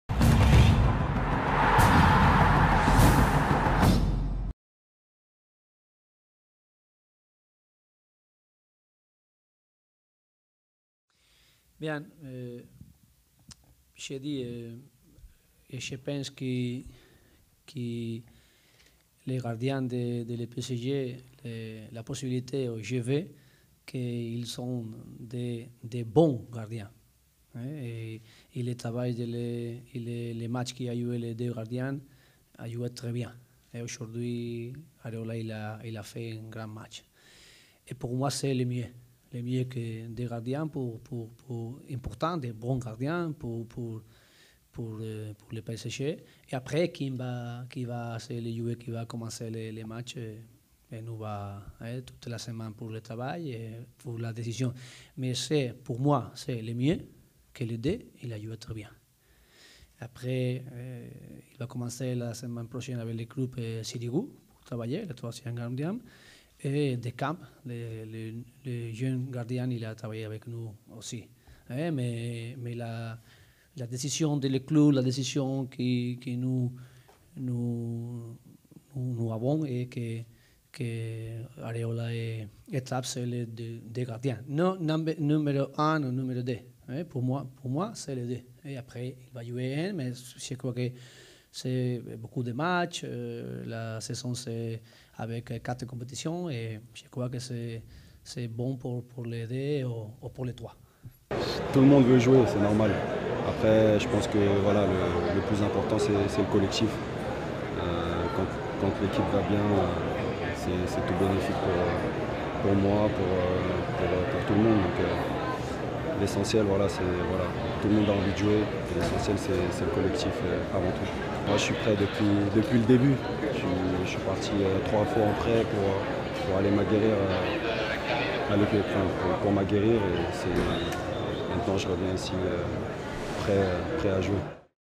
Aréola ou Trapp ? La bataille des gardiens continue au PSG
Qui entre Kevin Trapp et Alphonse Aréola sera gardien titulaire dans les cages du Paris Saint-Germain ? Unai Emery refuse encore de trancher.